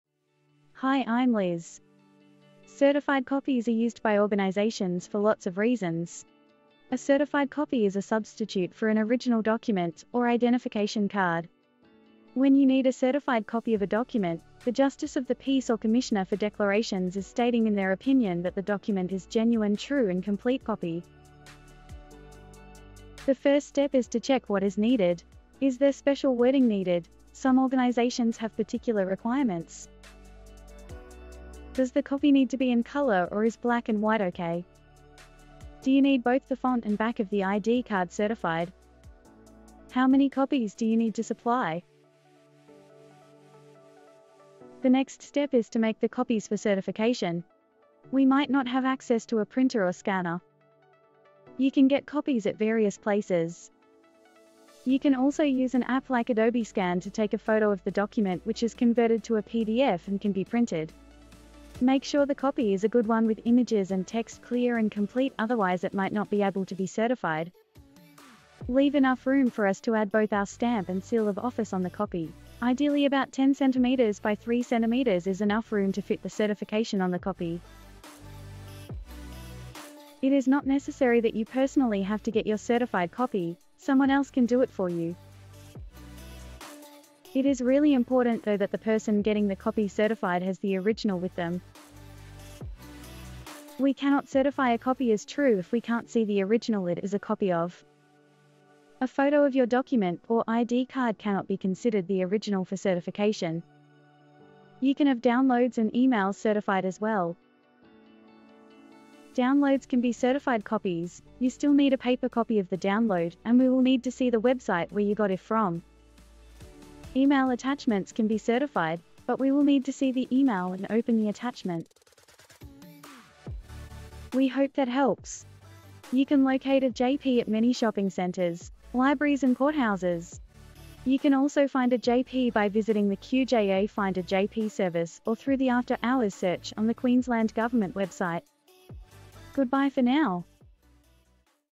Getting a Certified Copy
Need to provide a certified copy - this is what you need to know before seeing a JP to certify a copy for you.